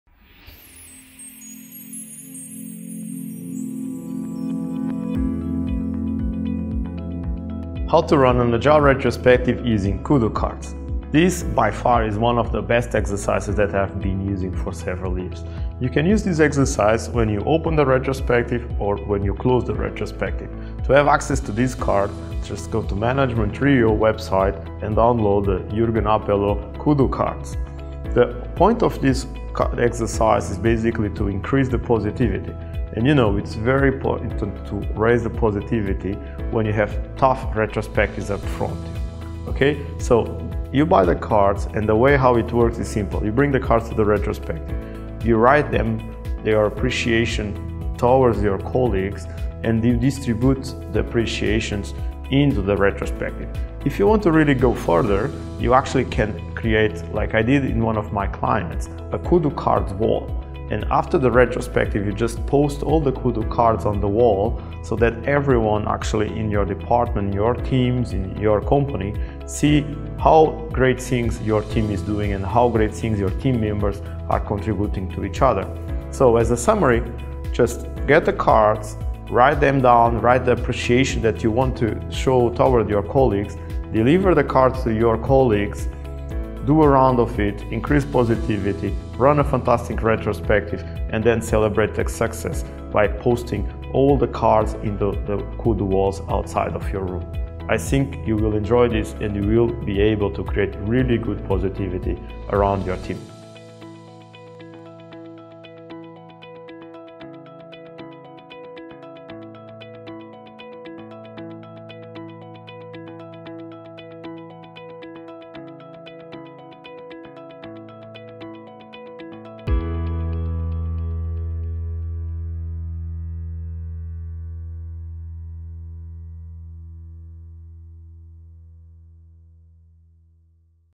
Agile Retrospectives: Kudo Cards Exercise
In this video, I will explain how can you use The Kudo Cards Retrospective Exercise as a tool for your retrospective.